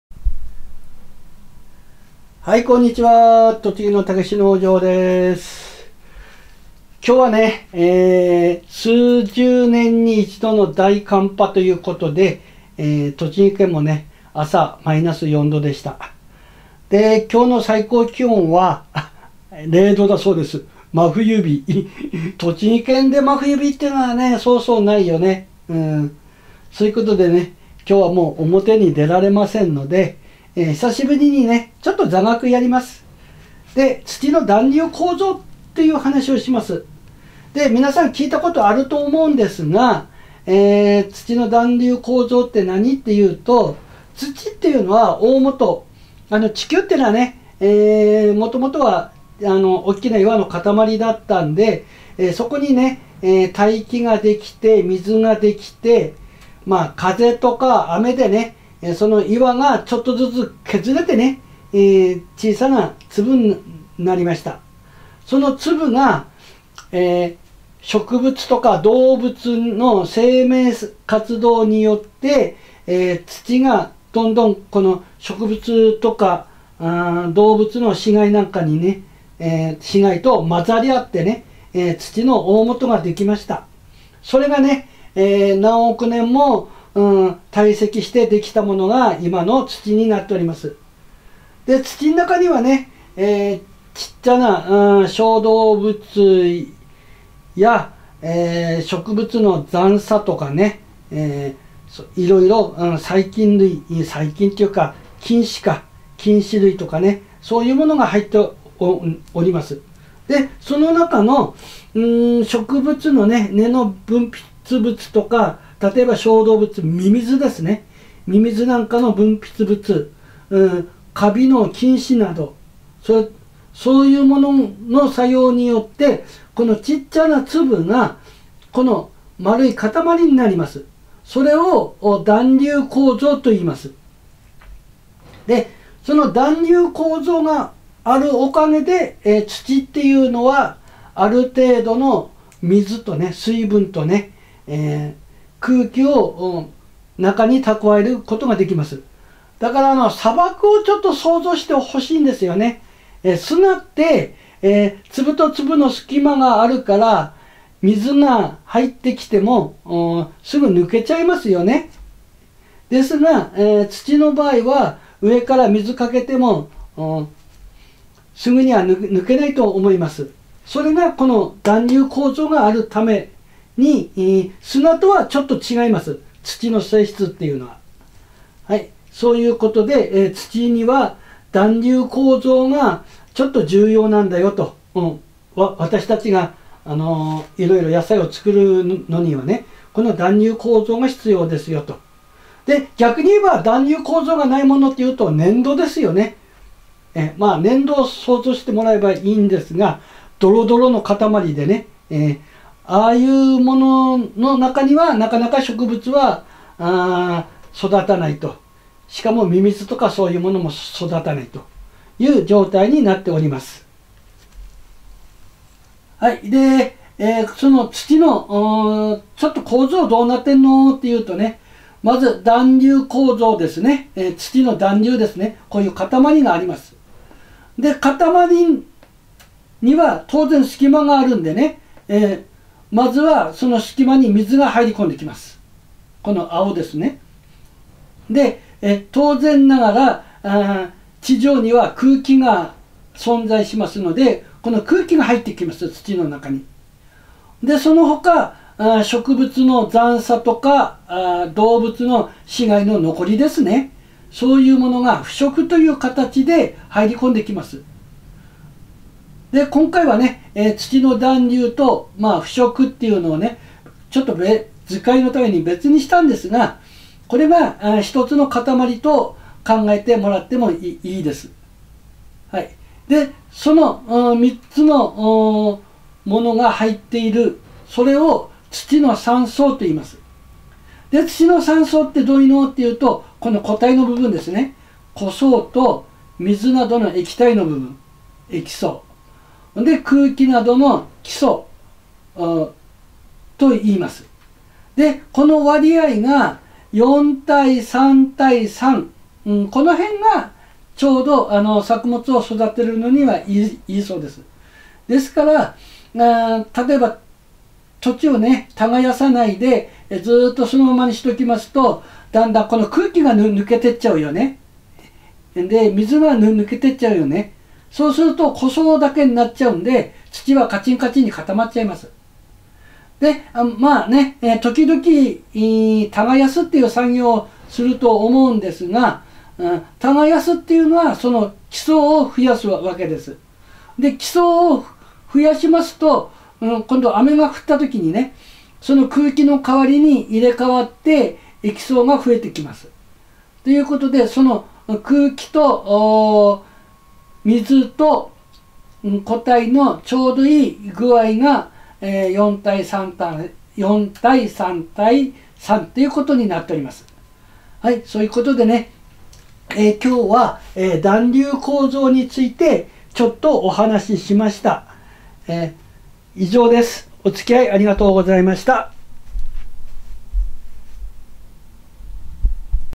土の団粒構造とは. 23/01/27 Vol.3274
今日は栃木の武子農場です。土の団粒構造について解説動画です。